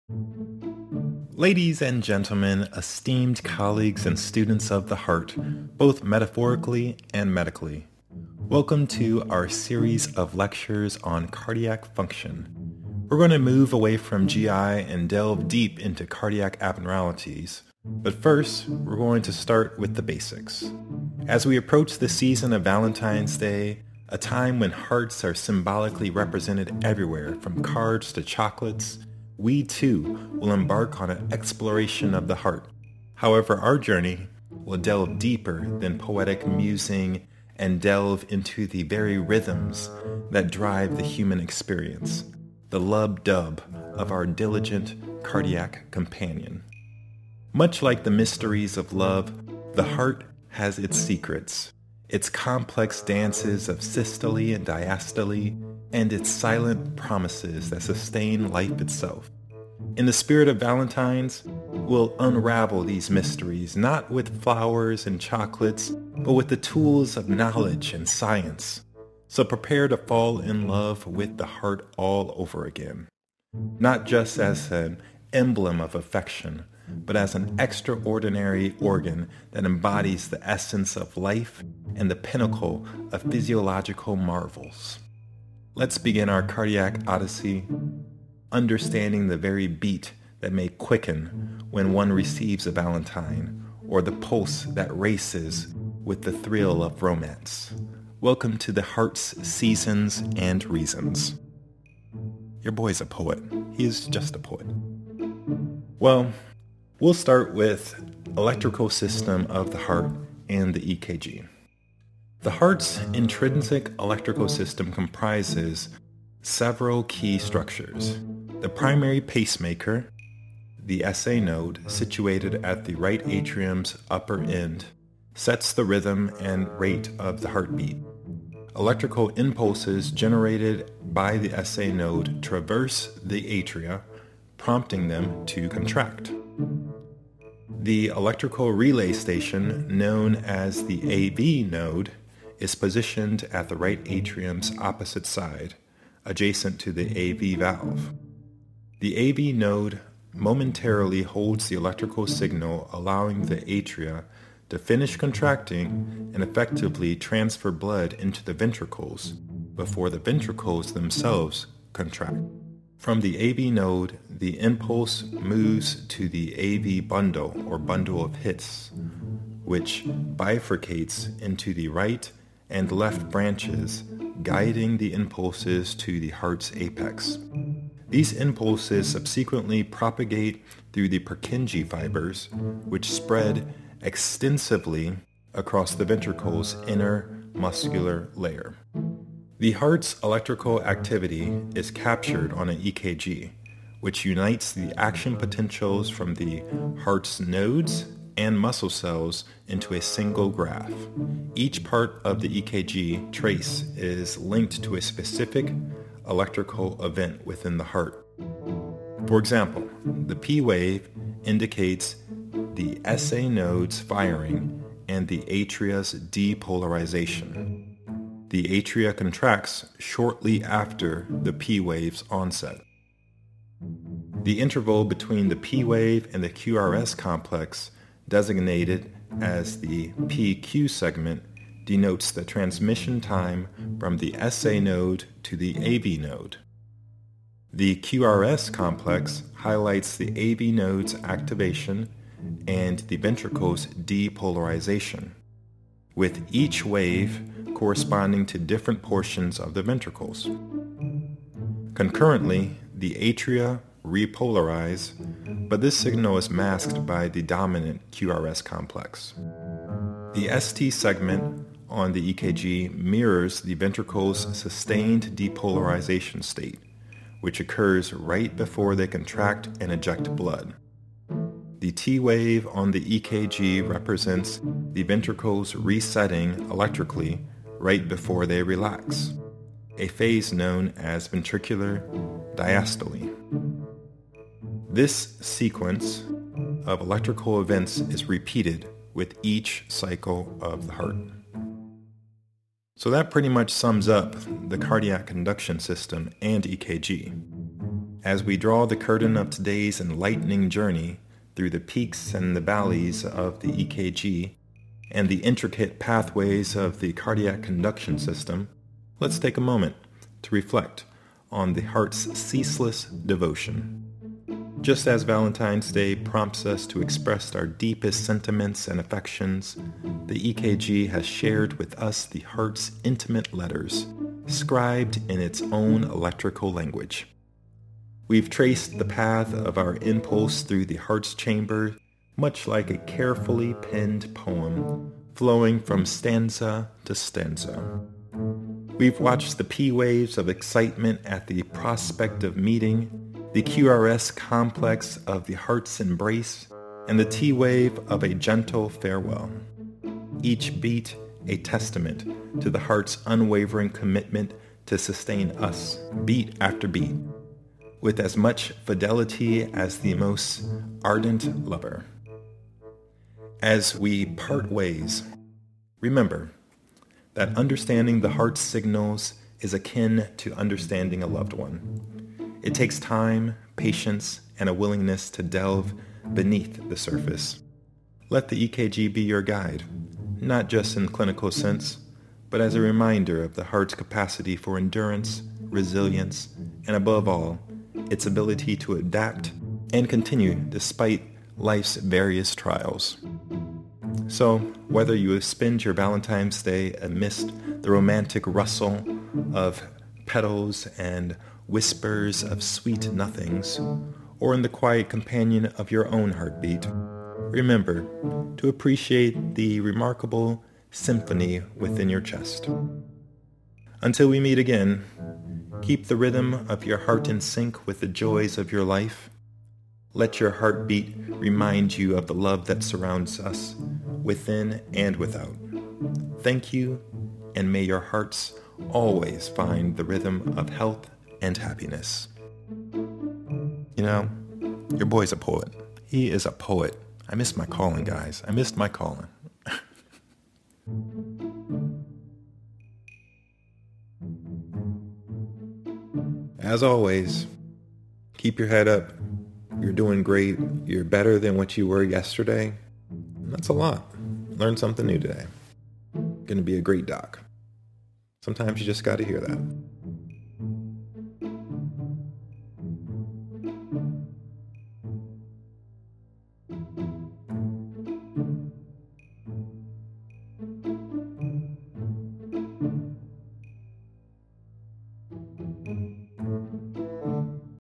Unravelling the Cardiac Conduction System and Understanding the EKG
Welcome to the funniest crash course in heartbeats and squiggly lines you'll ever attend! Today, we're diving into the electric dance of the heart and those wild EKG rhythms. Picture your heart as the ultimate DJ, dropping beats with precision. But sometimes, it gets a little overzealous, throwing in extra beats like it's trying to start a solo dance party in your chest.
Now, grab an EKG strip. You might think it's a seismograph reading from a minor earthquake, but nope, it's the heart's way of sliding into your DMs, saying, "Hey, look at me!" Reading an EKG is like decoding a secret language, only instead of emojis, we've got peaks and valleys telling us the juicy gossip from your ticker.
So if your heart skips a beat, don't panic. It might just be freestyling. And if your EKG looks like a rave in full swing, maybe, just maybe, it's time to chill. Buckle up for a ride through the heart's electric boogaloo – let's get this party started!

Lecture Text for your reading enjoyment :)

The heart's intrinsic electrical system comprises several key structures. The primary pacemaker, the SA node situated at the right atrium's upper end, sets the rhythm and rate of the heartbeat. Electrical impulses generated by the SA node traverse the atria, prompting them to contract.
The electrical relay station, known as the AV node, is positioned at the right atrium's opposite side, adjacent to the AV valve. The AV node momentarily holds the electrical signal, allowing the atria to finish contracting and effectively transfer blood into the ventricles before the ventricles themselves contract.
From the AV node, the impulse moves to the AV bundle, or Bundle of His, which bifurcates into right and left branches guiding the impulses to the heart's apex. These impulses subsequently propagate through the Purkinje fibers, which spread extensively across the ventricles' inner muscle layer.
The heart's electrical activity is captured on an ECG, which unites the action potentials from the heart's nodes and muscle cells into a single graph. Each part of the ECG trace is linked to specific electrical events within the heart. The P wave indicates the SA node's firing and the atria's depolarization. The atria contract shortly after the P wave's onset.
The interval between the P wave and the QRS complex, designated as the P-Q segment, denotes the transmission time from the SA node to the AV node. The QRS complex highlights the AV node's activation and the ventricles' depolarization, with each wave corresponding to different portions of the ventricles. Concurrently, the atria repolarize, but this signal is masked by the dominant QRS complex.
The S-T segment on the ECG mirrors the ventricles' sustained depolarization state, which occurs right before they contract and eject blood. The T wave on the ECG represents the ventricles resetting electrically right before they relax, a phase known as ventricular diastole. This sequence of electrical events is repeated with each cycle of the heart.